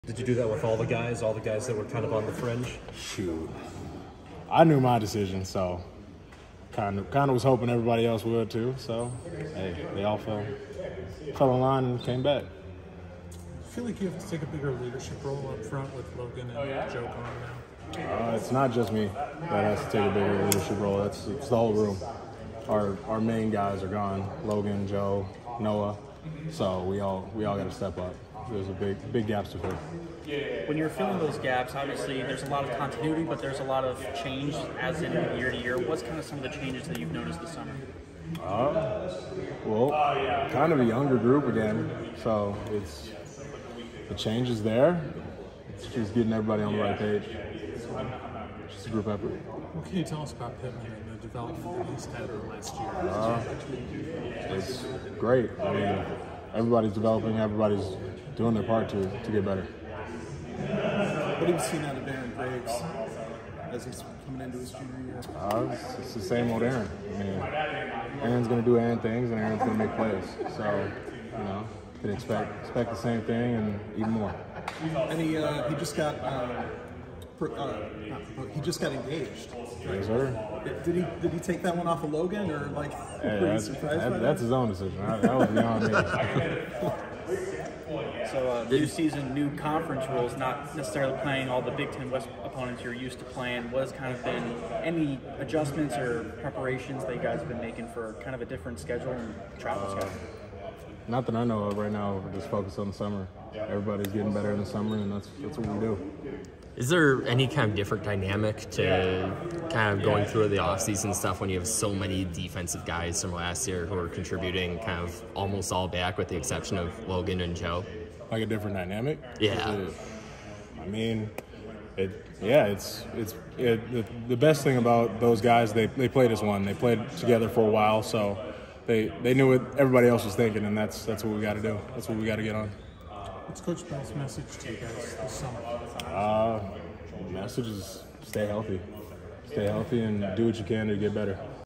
Yahya Black talks about Iowa's returning defensive linemen going into the 2024 season
Iowa DL Yahya Black answered questions during an offseason media session June 18 in Iowa City.